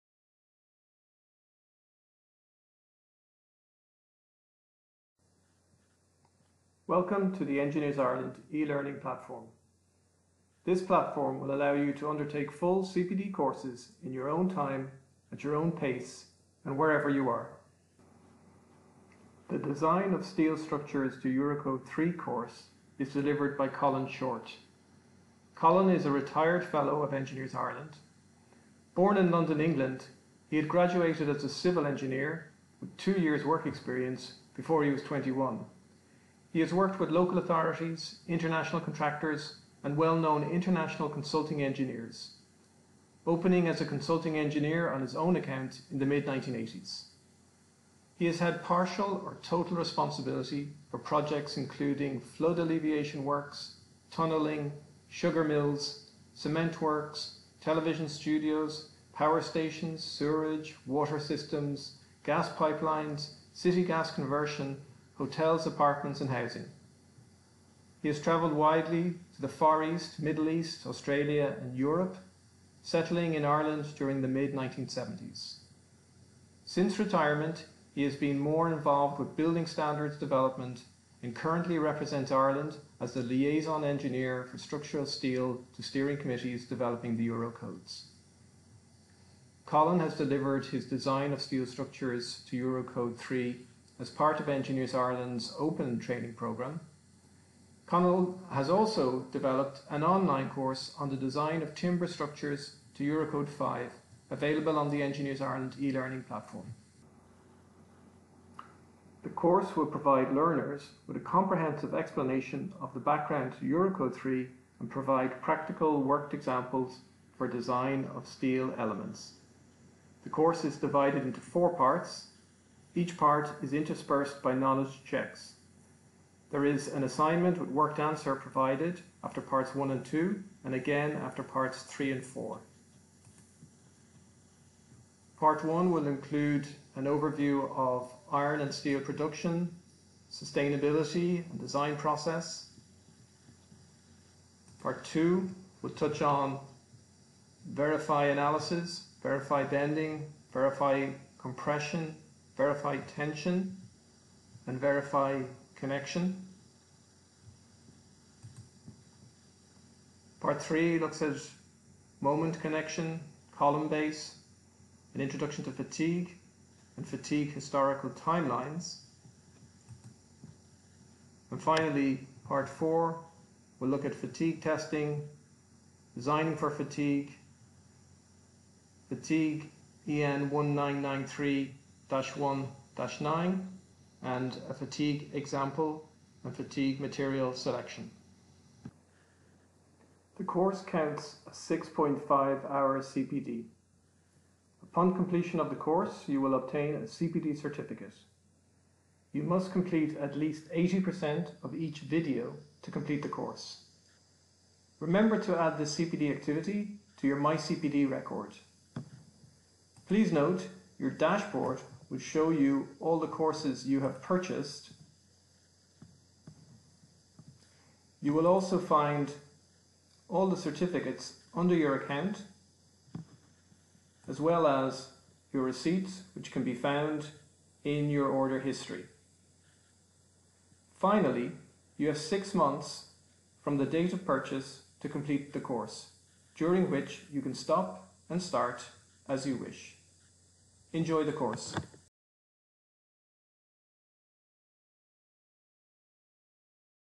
Design of Steel Structures | Engineers Ireland eLearing Course Preview
Engineers Ireland has developed a selection of CPD courses that are available as eLearning courses that can be taken any time, anywhere and at your own pace.
Design of Steel Structures to Eurocode 3
This course takes the learner through the design of steel structures to Eurocode 3.